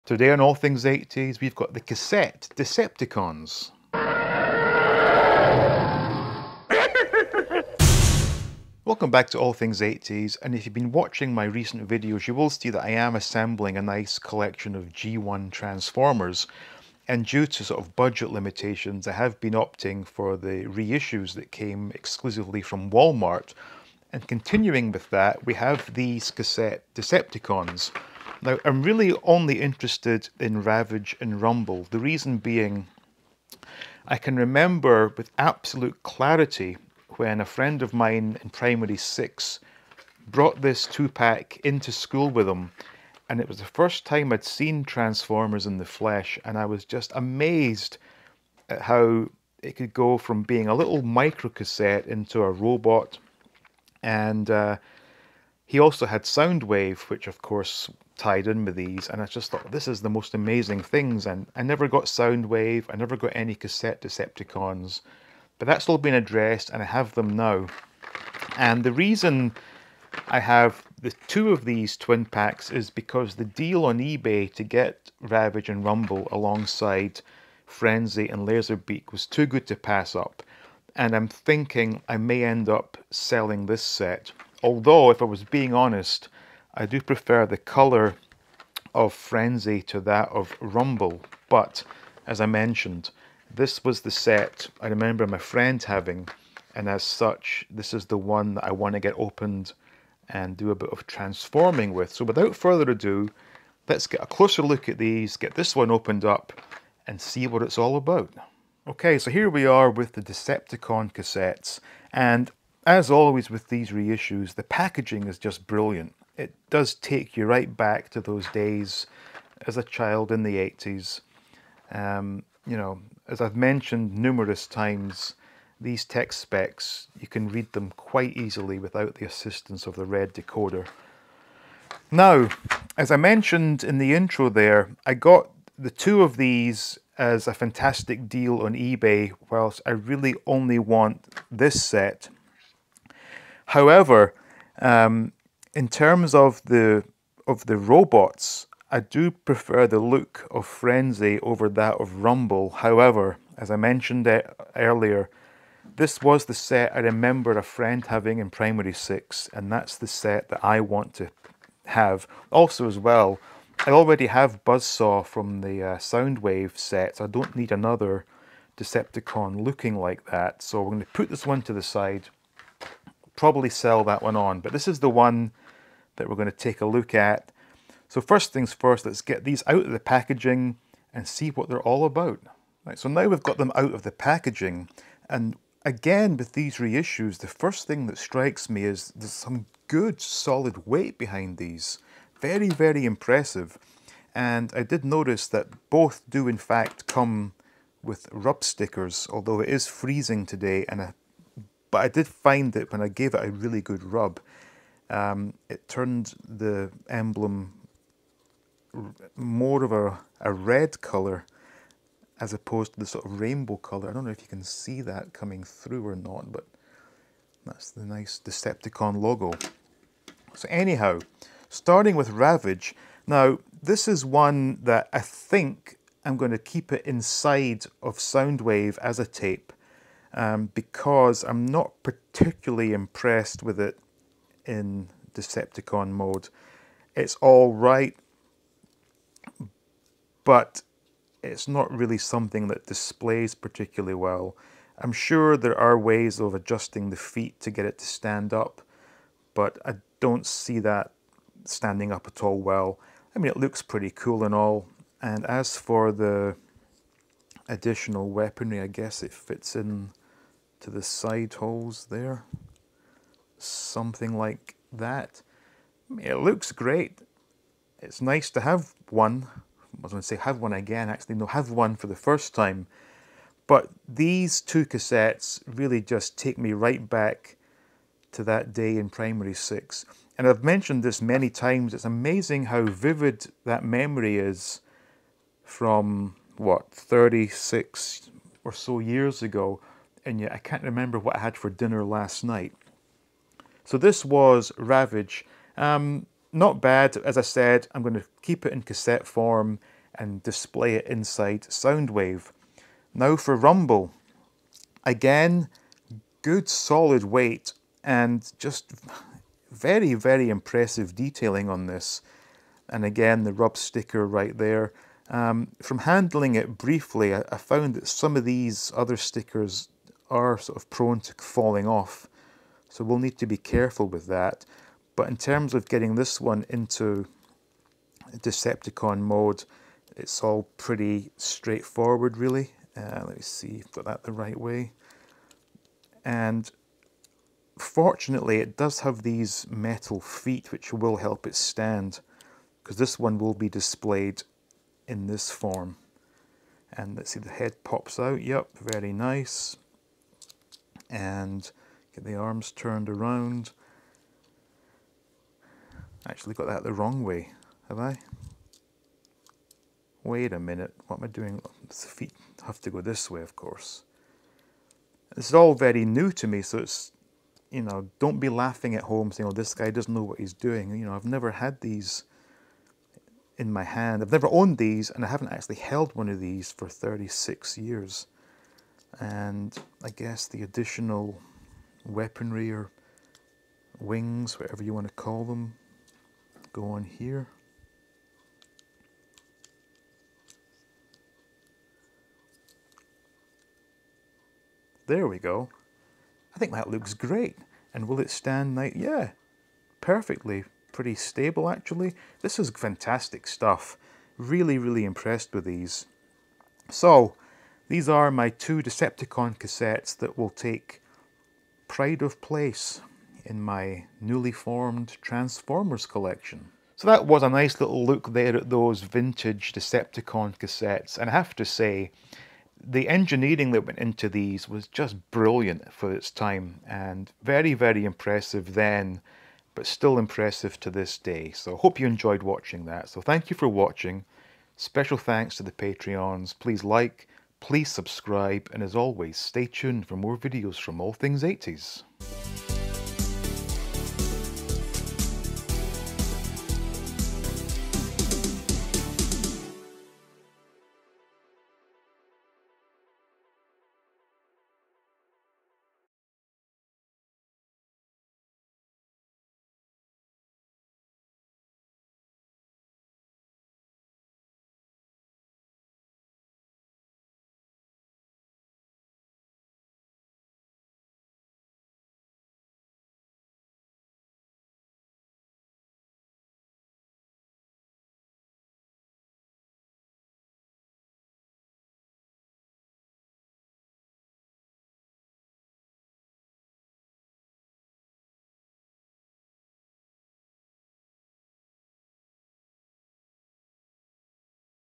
G1 TRANSFORMERS CASSETTE DECEPTICONS - unboxing & review
I've had my eye on  these since the 80s, so when I got the chance to get a G1 reissue, I jumped at the opportunity.